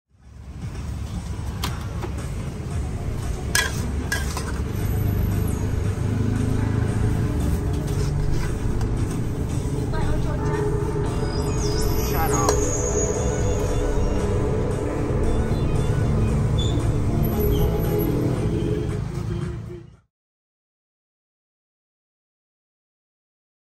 Spicy Fry Rich with herb#short#asian food#streetfood
hello everyone happy to tell you and easy fry rice and faster when you dont have enough time. just watch this videos will understand.